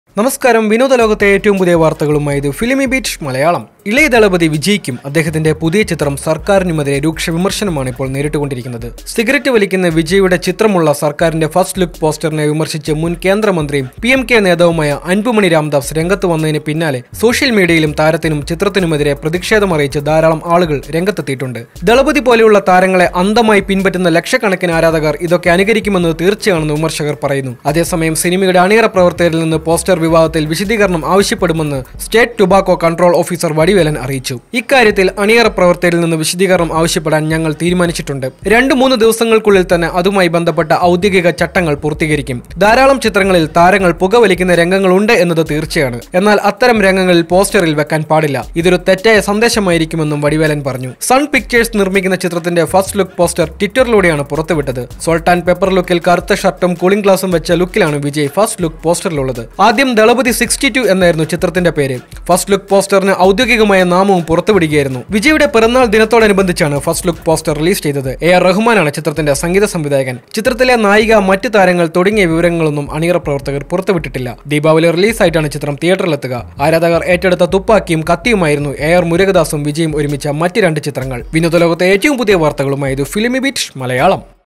‘സര്‍ക്കാര്‍’ പോസ്റ്ററിനെതിരെ രൂക്ഷവിമര്‍ശനം | filmibeat Malayalam
ദളപതി വിജയ്ക്കും അദ്ദേഹത്തിന്റെ പുതിയ ചിത്രം സര്‍ക്കാരിനുമെതിരെ രൂക്ഷവിമര്‍ശനം. സിഗരറ്റ് വലിക്കുന്ന വിജയുടെ ചിത്രമുള്ള സര്‍ക്കാരിന്റെ ഫസ്റ്റ്‌ലുക് പോസ്റ്ററിനെ വിമര്‍ശിച്ച് മുന്‍ കേന്ദ്രമന്ത്രിയും പി എം കെ നേതാവുമായ അന്‍പുമണി രാം ദാസ് രംഗത്തുവന്നതിനു പിന്നാലെ സോഷ്യല്‍ മീഡിയയിലും താരത്തിനും ചിത്രത്തിനുമെതിരെ പ്രതിഷേധമറിയിച്ച് ധാരാളം ആളുകള്‍ രംഗത്തെത്തിയിട്ടുണ്ട്. vijay sarkkar poster contraversy